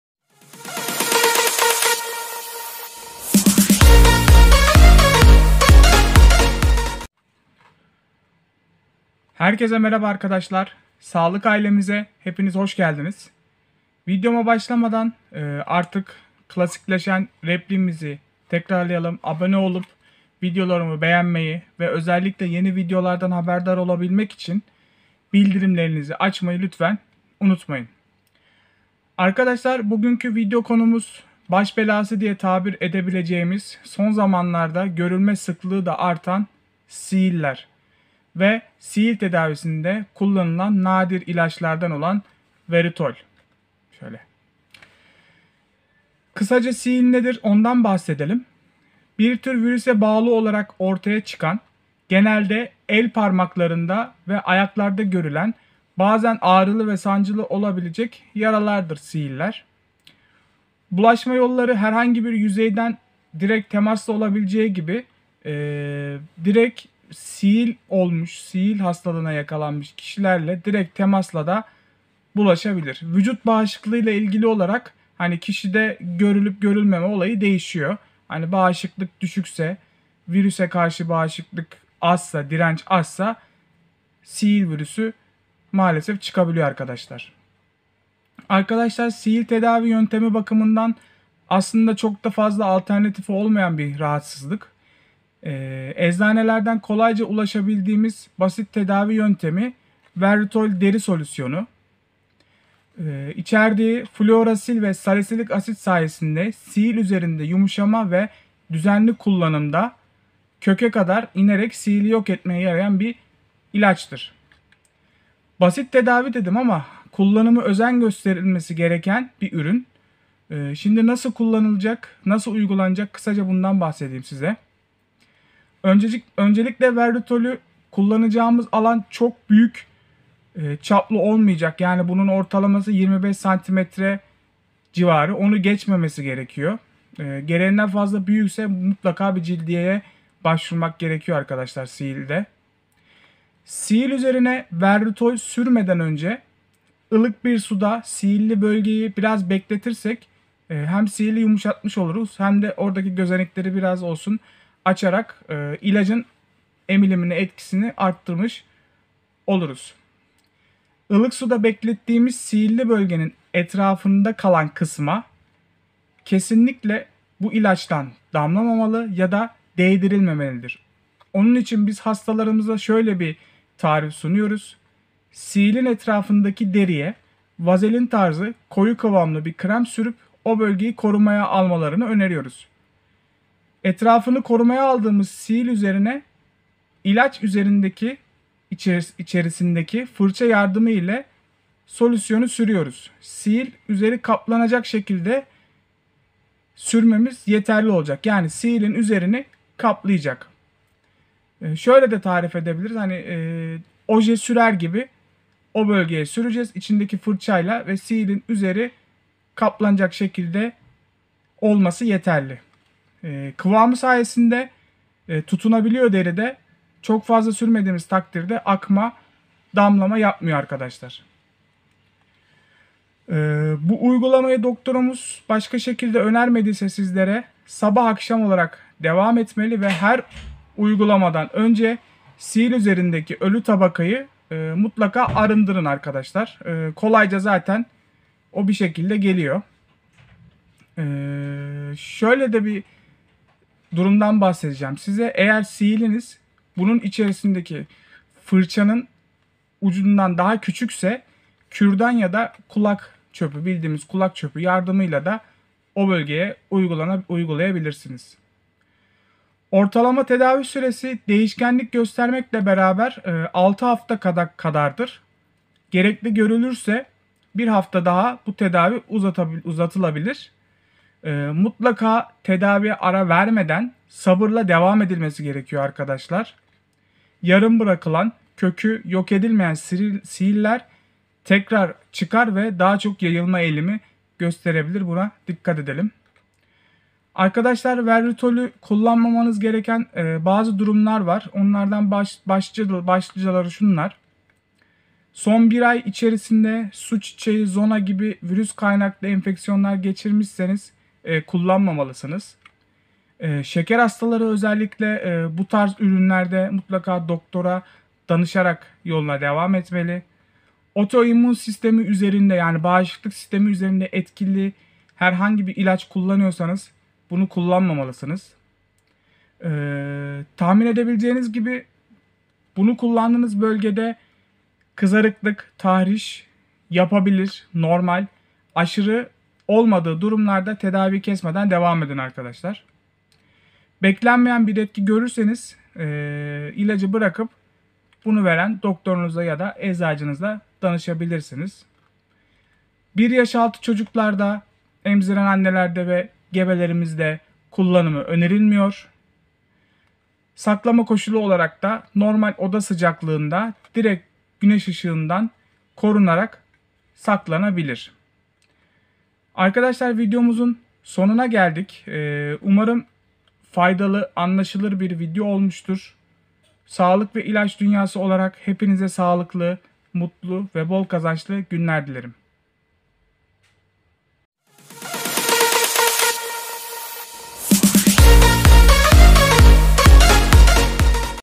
siğil tedavisinde verrutol kullanımı mutlaka izleyin
arkadaşlar bugünkü videomda baş belası siğillerden kurtulma yolları nelerdir onlardan bahsettim

verrutol solüsyon nedir nasıl kullanılır
siğillerden kurtulma yöntemleri nelerdir
siğil tedavisi nasıl olur